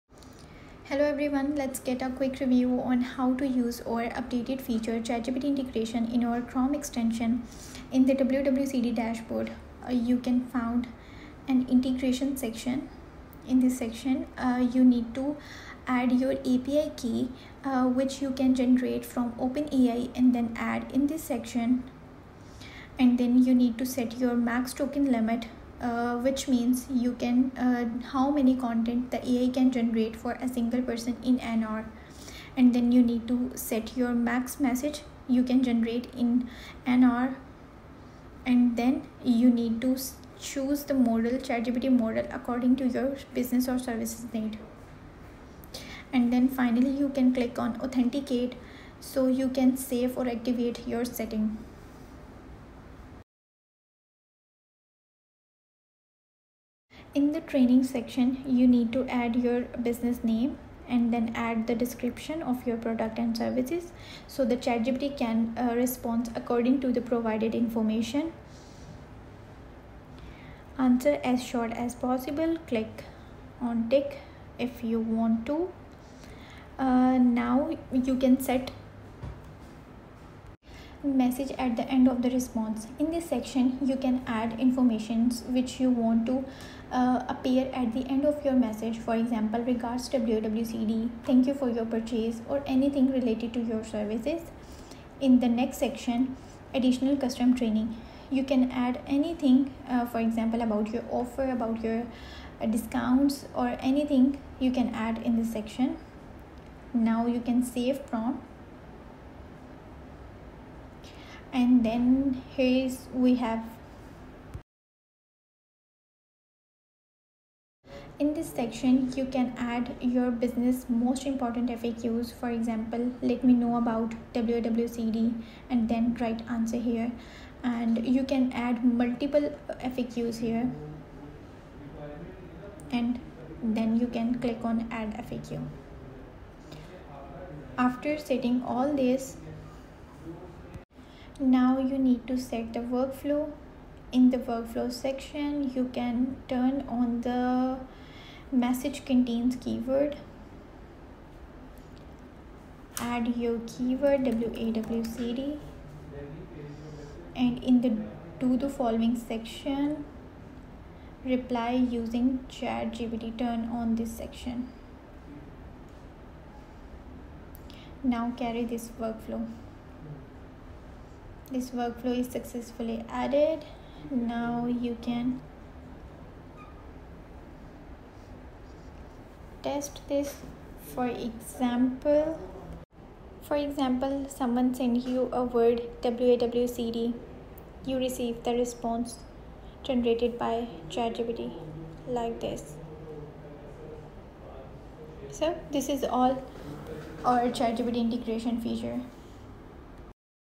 How to use ChatGPT to Automate WhatsApp Messages | ChatGPT WAWCD Integration
Learn how to integrate ChatGPT into WAWCD in this quick demo! From API setup to training, workflows, and FAQs - see how to activate AI-powered responses step by step.